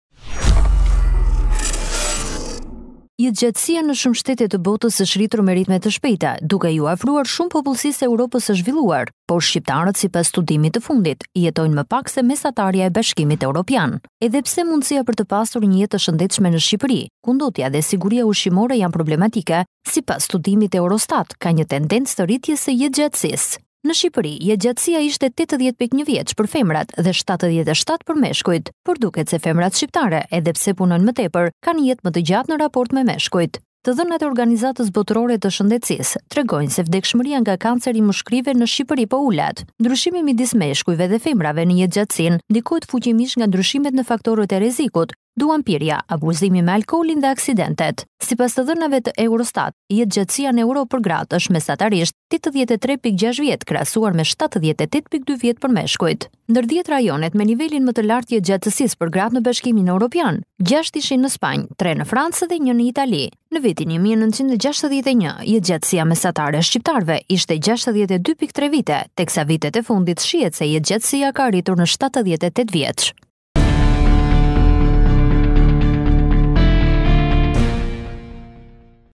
Sa jetojnë shqiptarët? Eurostat: Më pak vite, se mesatarja evropiane | IN TV Albania
Jetëgjatësia në shumë shtete të botës është rritur me ritme të shpejta, duke iu afruar shumë popullsisë së Evropës së zhvilluar, por shqiptarët sipas studimit të fundit jetojnë më pak se mesatarja e BE-së. Edhe pse mundësia për të pasur një jetë të shëndetshme në Shqipëri, ku ndotja dhe sigurisë ushqimore janë problematike, sipas studimit të Eurostat ka një tendencë të rritjes së jetëgjatësisë. Jetëgjatësia në Shqipëri ishte 80.1 vjeç për femrat dhe 77 vjeç për meshkujt. Por, duket se femrat shqiptare edhe pse punojnë më shumë, kanë jetë më të gjatë në raport me meshkujt. Të dhënat e OBSH-së tregojnë, se vdekshmëria nga kanceri i mushkërive në Shqipëri po ulet. Ndryshimi midis meshkujve dhe femrave në jetëgjatësinë ndikohet fuqimisht nga ndryshimet në faktorët e rrezikut, duhanpirja, abuzimit me alkoolin dhe aksidentet. Sipas të dhënave të Eurostat, jetëgjatësia në Evropë për gratë është mesatarisht 83.6 vjet, krahasuar me 78.2 vjet për meshkujt. Ndër 10 rajonet me nivelin me të lartë të jetëgjatësisë për gratë në BE, gjashtë ishin në Spanjë, tre në Francë dhe një në Itali. Në vitin 1961 jetëgjatësia mesatare e shqiptarëve ishte 62.3 vite, teksa vitet e fundit shihet se jetëgjatësia ka rritur në 78 vjeç.